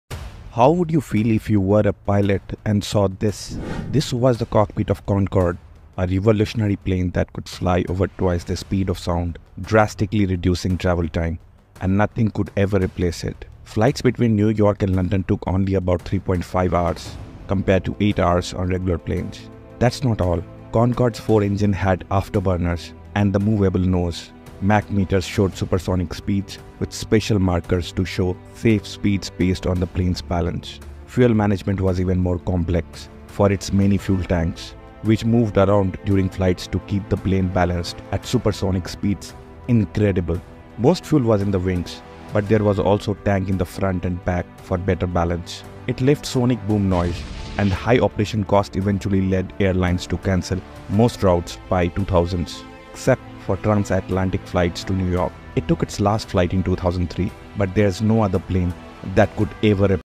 Concorde's Cockpit Was Insane x😲history
Description: 🚀 Step inside the legendary Concorde cockpit! This groundbreaking aircraft flew over twice the speed of sound, cutting travel time between New York and London to just 3.5 hours. 🌍✈️ Discover the incredible features that made the Concorde unique, from its four afterburner engines to its moveable nose and complex fuel management system. Don’t miss this look into aviation history!